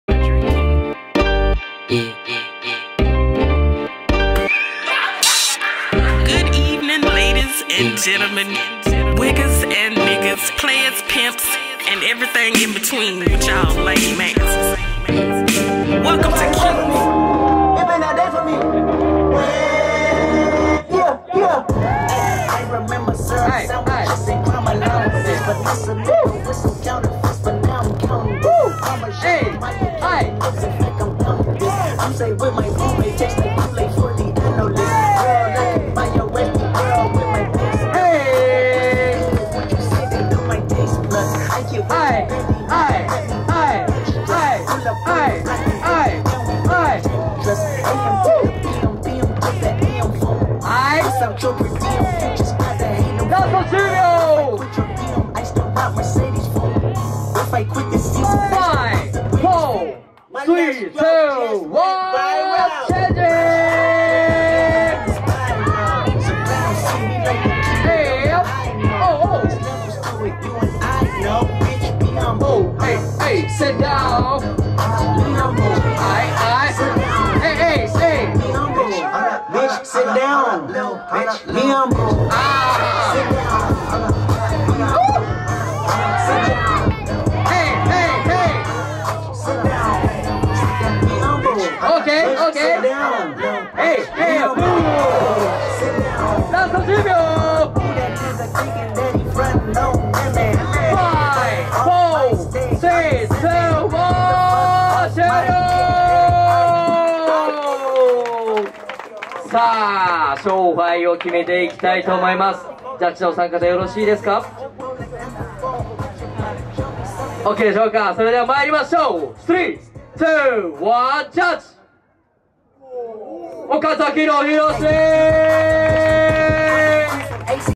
BattleONEWAY in SHINOJIMA Best16【ChildTwiggzVS岡崎のヒロシ】
BattleONEWAY in Shinojima
Dance freestyle battle!
The place is Aichi Shinojima!